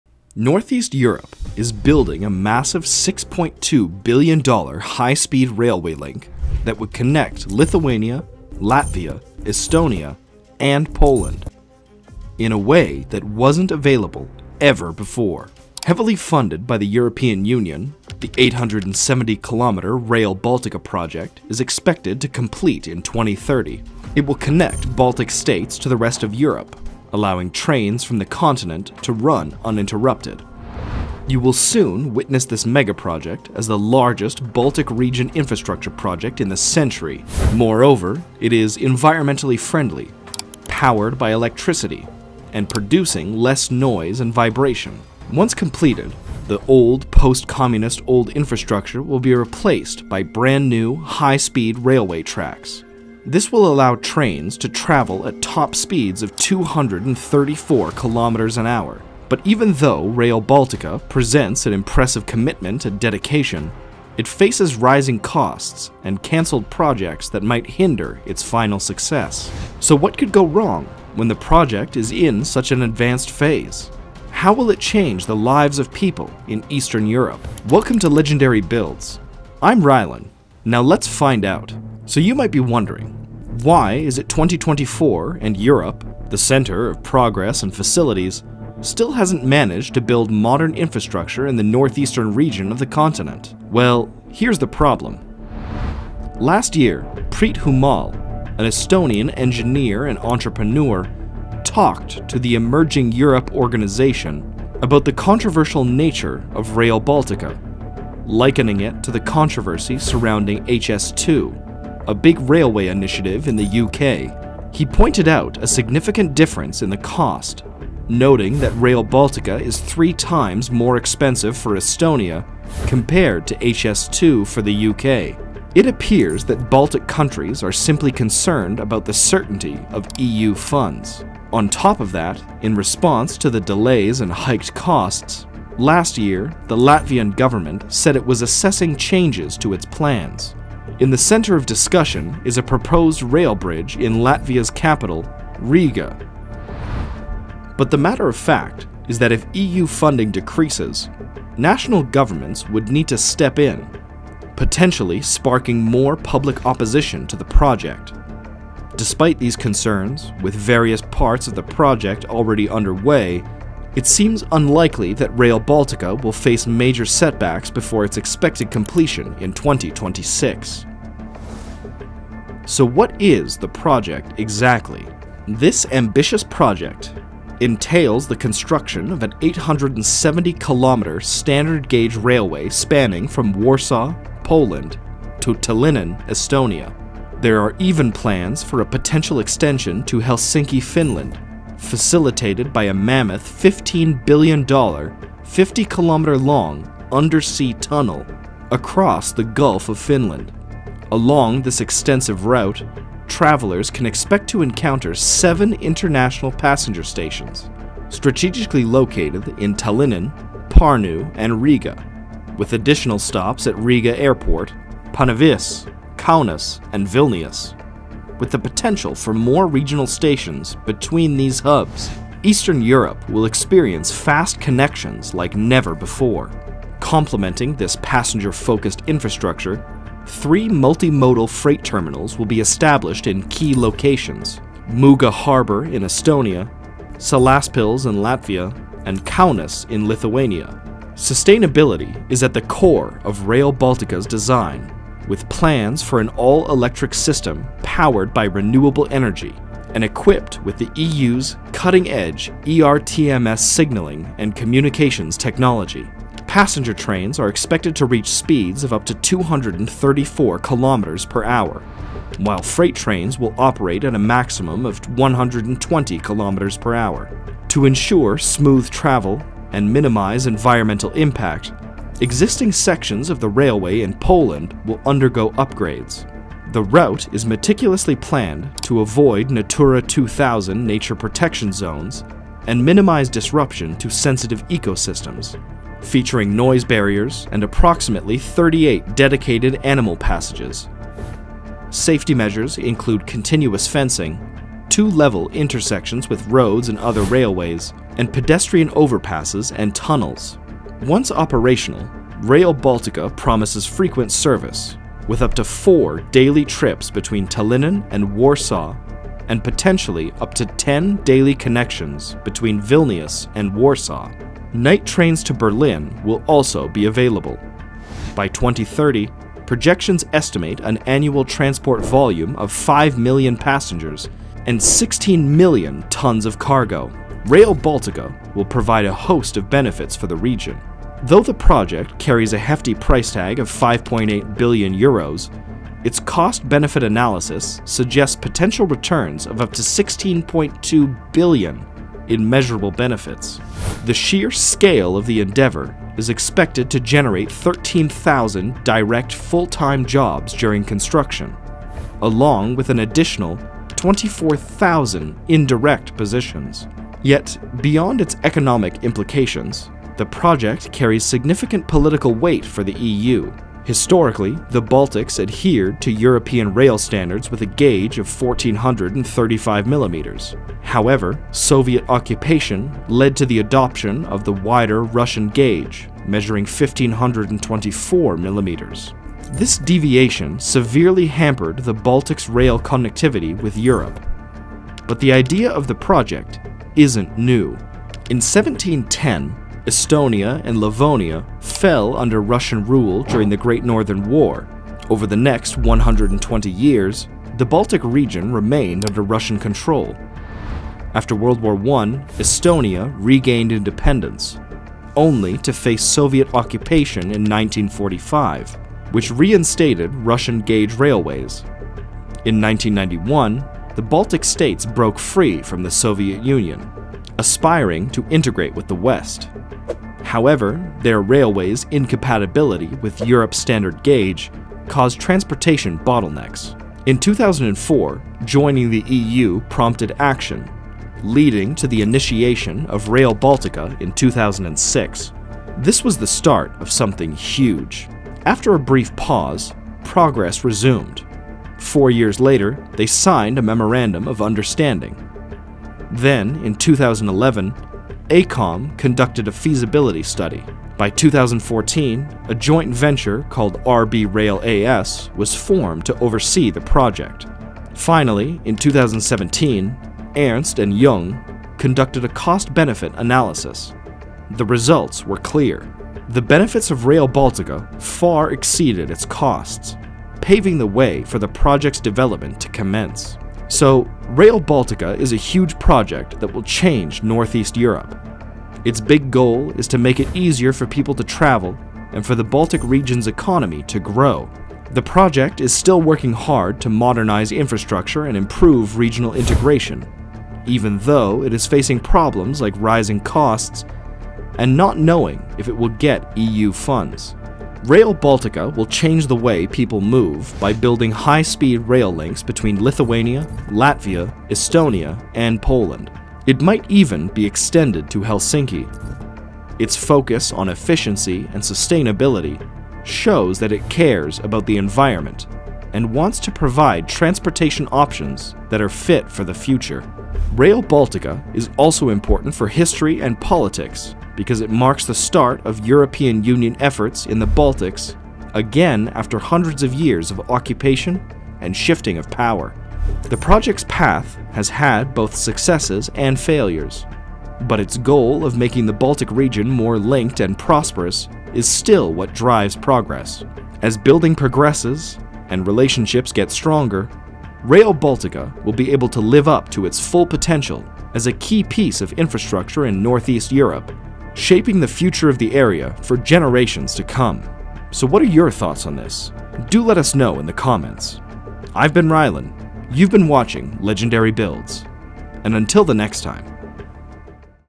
Inside The LARGEST Railway MegaProject In Eastern Europe
Today we will learn more about Europe's most revolutionary railway project in the Eastern part of the continent - The Rail Baltica. Welcome to Legendary Builds!
Chapters:
Intro: 0:00 - 1:20
Reasons: 1:20 - 2:50
Design: 2:50 - 5:56
Politics: 5:56 - 8:52
Summary: 8:52 - 9:27

Our Team:
Written by - Uzair
Narrated by - Ryllan
Edited by - Yousuf